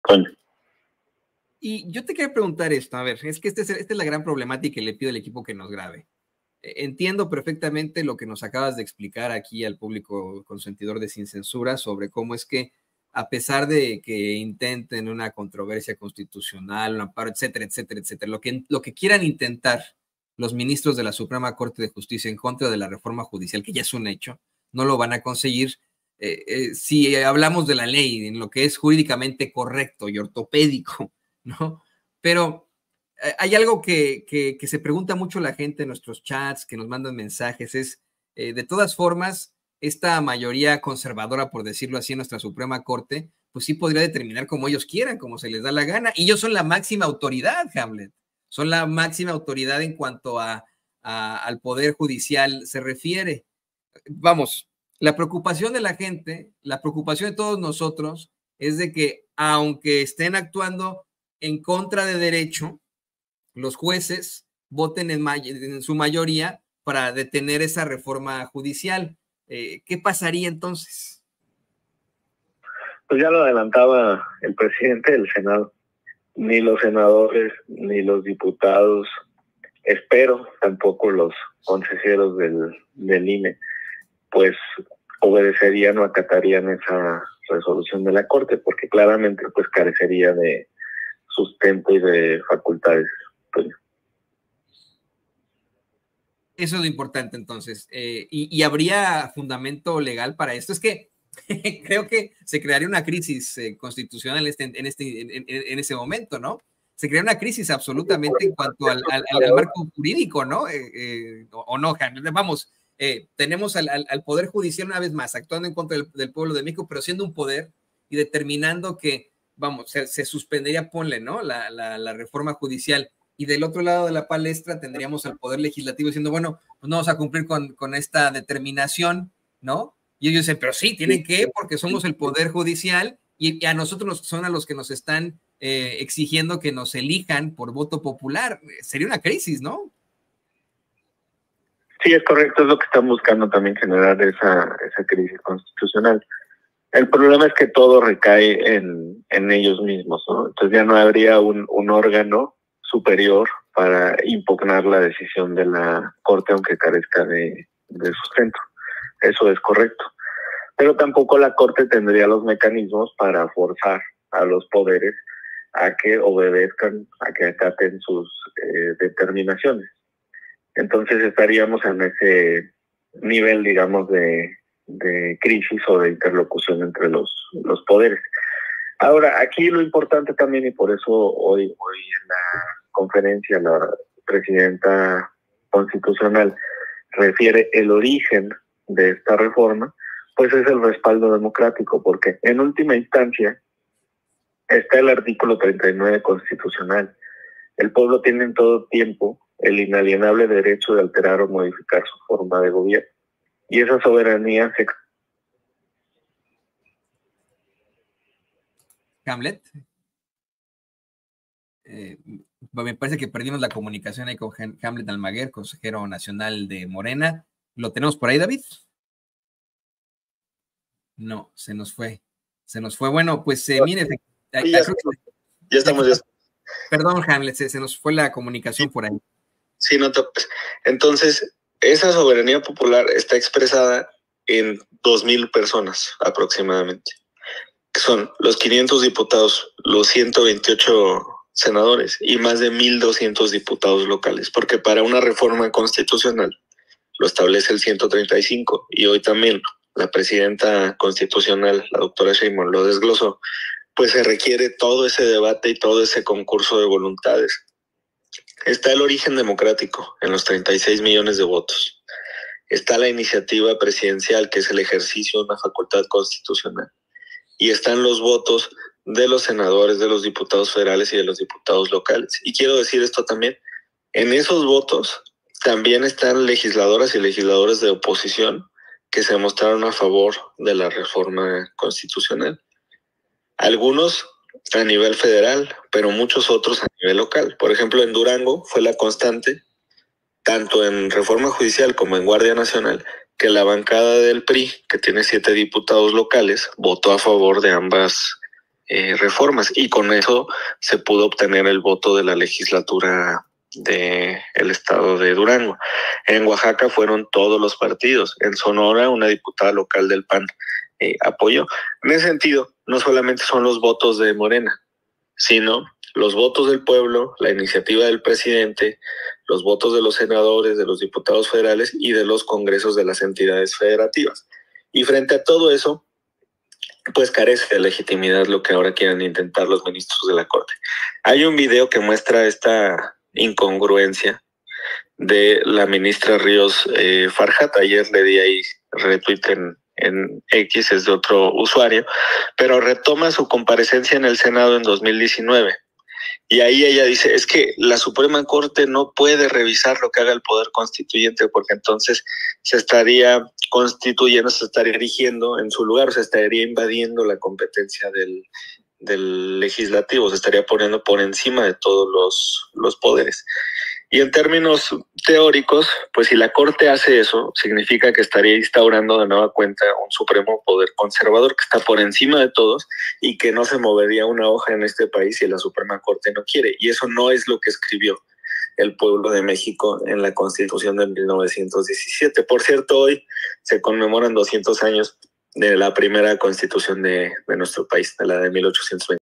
Sí podría haber una CRISIS si ministros determinan vs ReformaJudicial, advierte
Ante el supuesto de que esos ocho ministros cuestionables consigan determinar en contra de la ReformaJudicial, el exdiputado y ConsejeroNacional de morena hamletalmaguer nos comenta que ya se advirtió desde tanto la Cámara de Diputados como de la de Senadores que no acatarían tal determinación… Algo que efectivamente desencadenaría en una crisis. Claro, esto cuando la soberanía popular recae en los legisladores